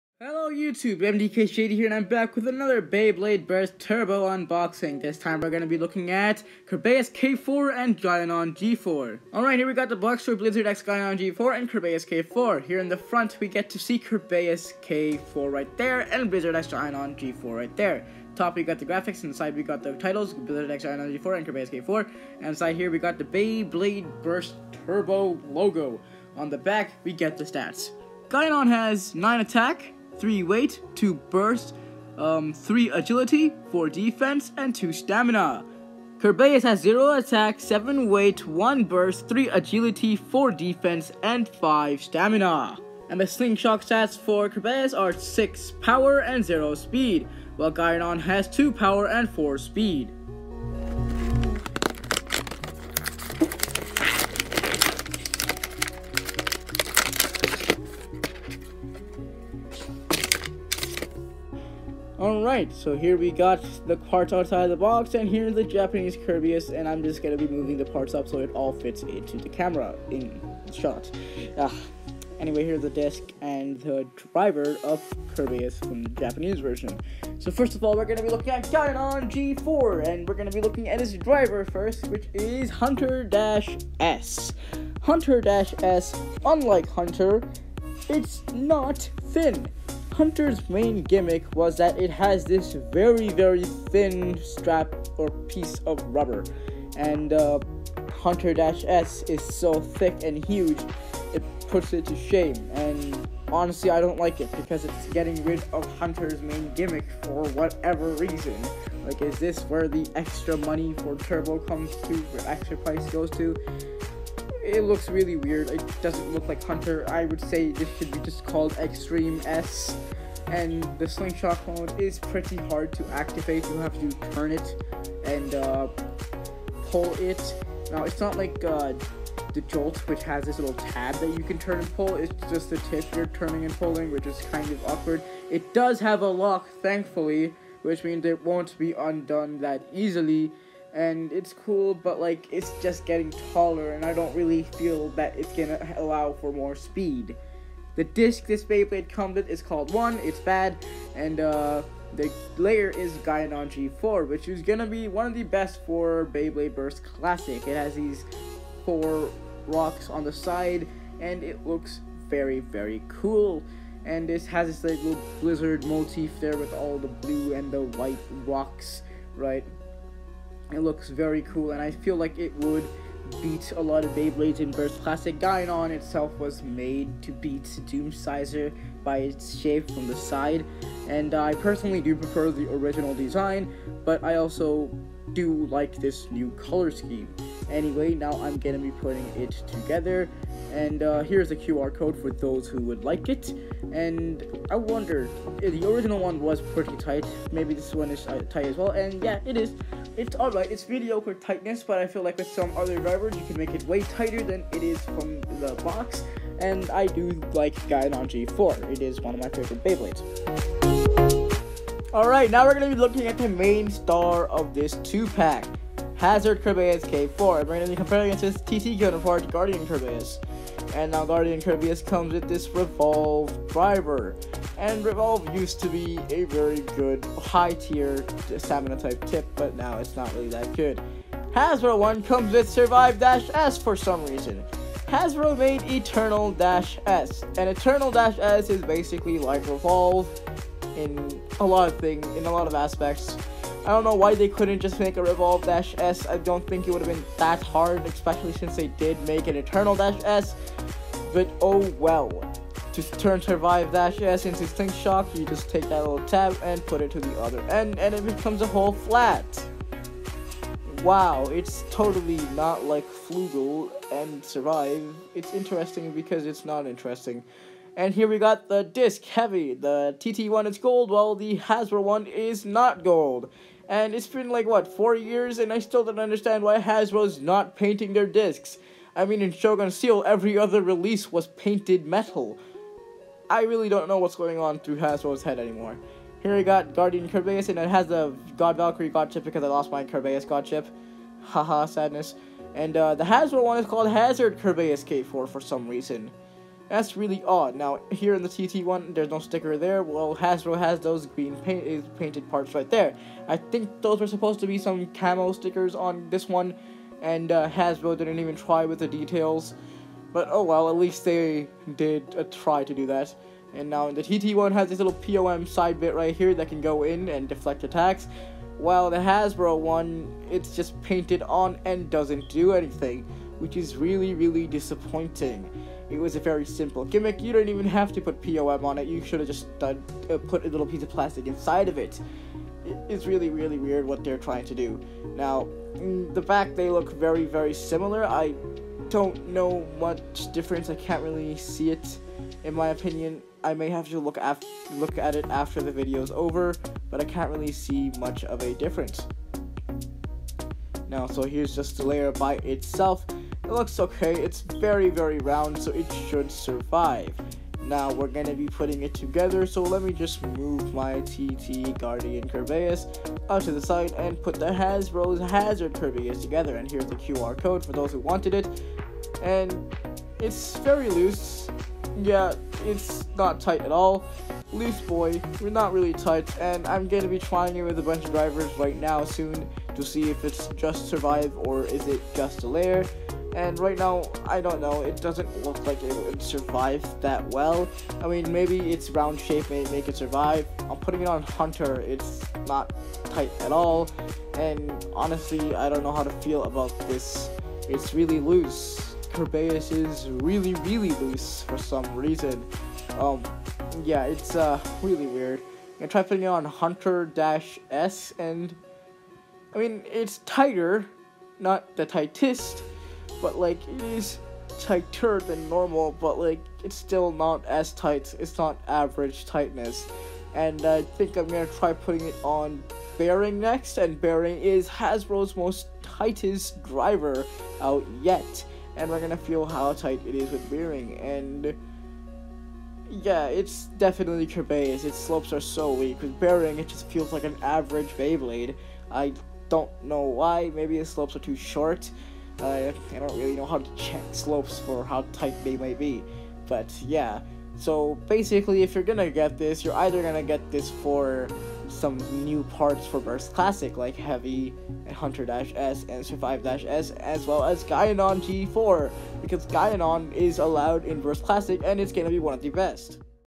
Beyblade Burst Turbo Unboxing & Review: Hazard Kerbeus K4 VS Blizzard X Gaianon G4
Hasbro calls Guardian Kerbeus, Hazard Kerbeus K4